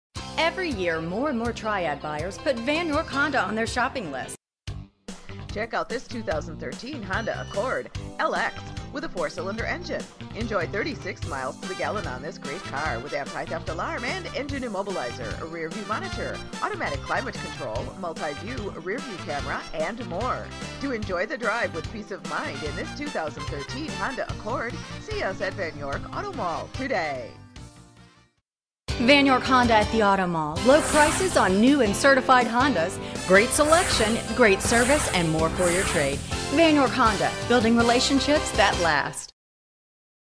Used 2013 Honda Accord ACCORD 4DR LX Greensboro, Winston Sal
New 2013 Honda Accord ACCORD 4DR LX Greensboro, Winston Salem High Point, Kernersville NC Vann, Check out this Alabaster Silvr 20 13 Honda Accord 4 Dr Sedan, equipped with a 4 Cyl. engine. Enjoy an exceptional 36 miles to the gallon on this great car with features like Remote Anti-Theft Alarm System With Engine Immobilizer, Electronic Brakeforce Distribution, Rear View Monitor, Front Air Conditioning With Automatic Climate Control, Multi-View Rear View Camera, and much more. Enjoy the drive and have peace of mind in this 20 13 Honda Accord. See us at Vann York Auto Mall today!

vin- 1HGCR2F33DA188739

422 Eastchester Dr.